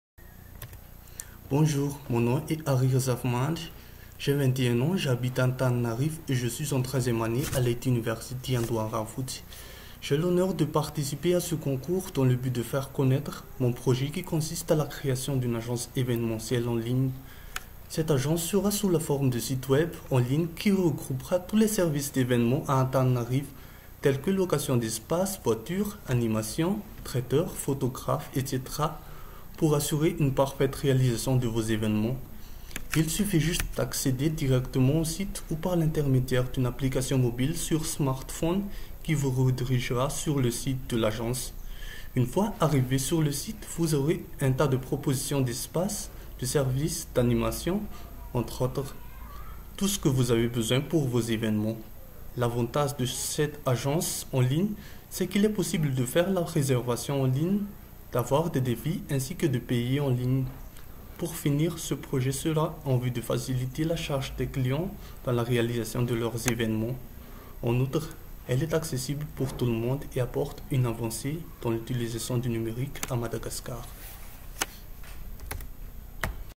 Event Repertor
Une agence événementielle en ligne en vue de faciliter la charge des personnes qui vont réaliser leurs événements . Le site regroupera tous les services d'événements à Antananarivo